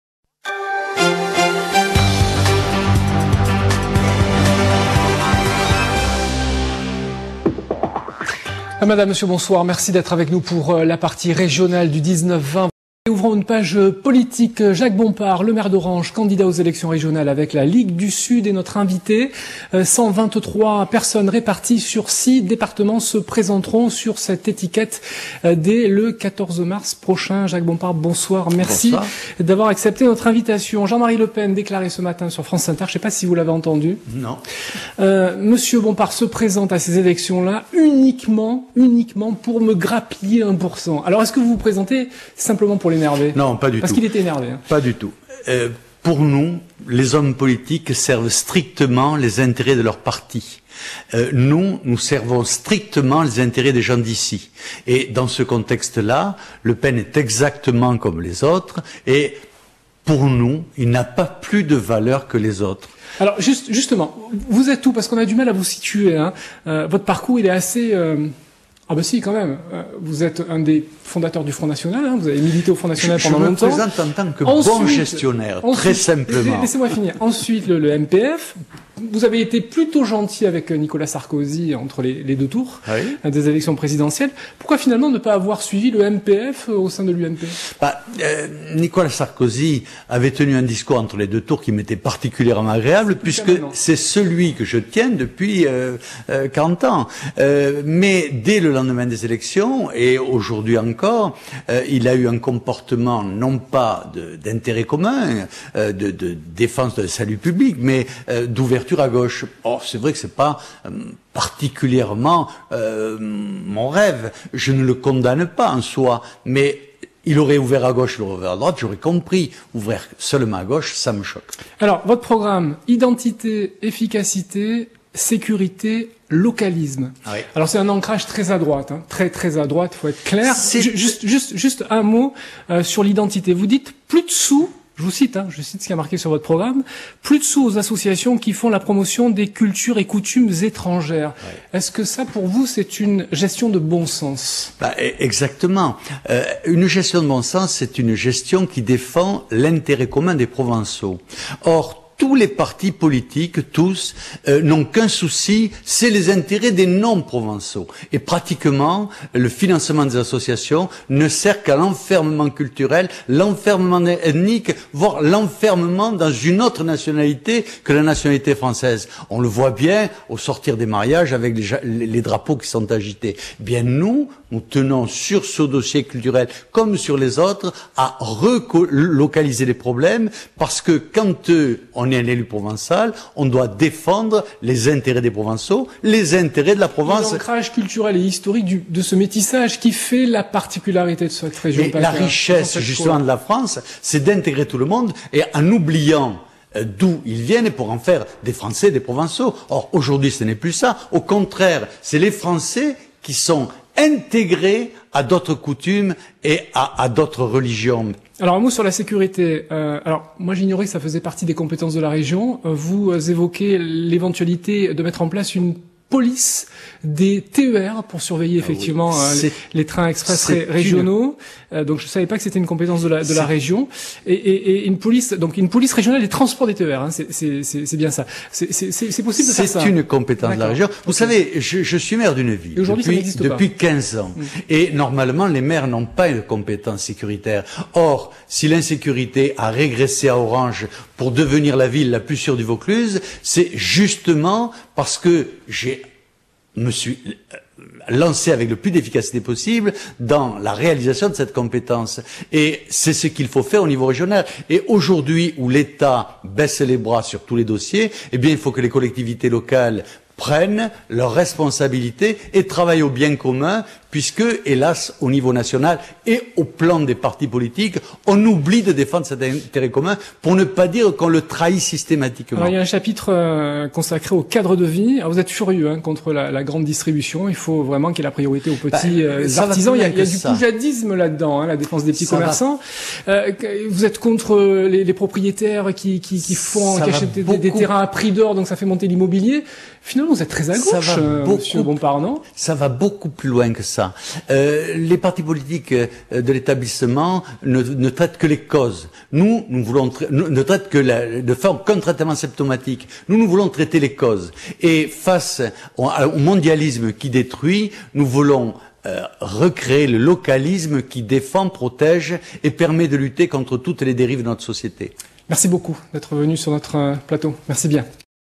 Jacques Bompard, Ligue du Sud : identité, efficacité, localisme
Jacques Bompard -tête de liste de la Ligue du Sud pour les élections régionales de mars 2010, maire de la ville d'Orange (Vaucluse)- était interviewé le 23 février 2010. Parmi les quatre volets de son programme (Identité, Sécurité, Efficacité, Localisme), les questions du journaliste l'on surtout amené à s'exprimer sur l'identité, la sécurité face à l'absence de volonté de l'état de l'assurer, le localisme protection contre la mondialisation.